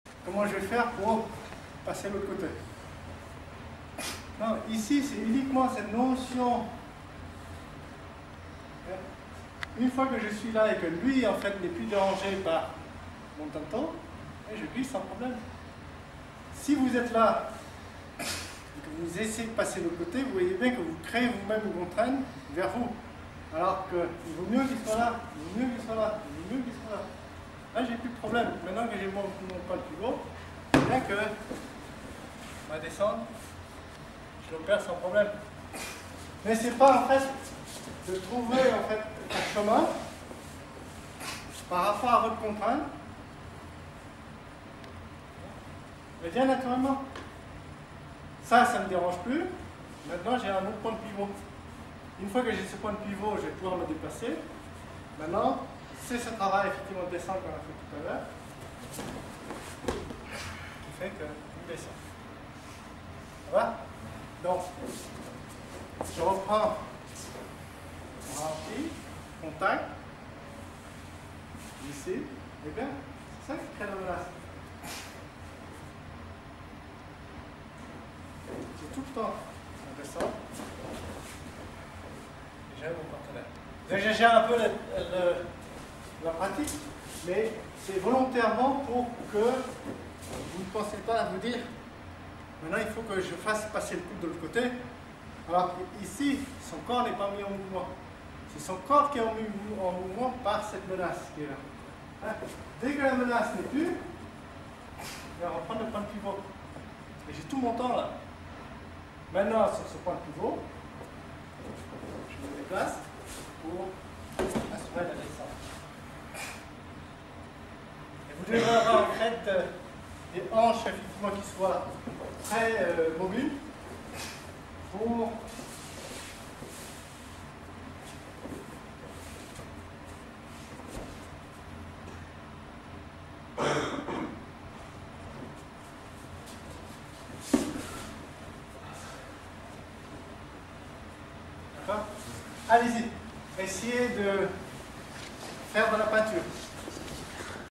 CMOM Aikido - 29.01.2009 - Jeudi de Varennes#11 7/9
Maurice Thai démontrant ikkyo omote avec un tanto.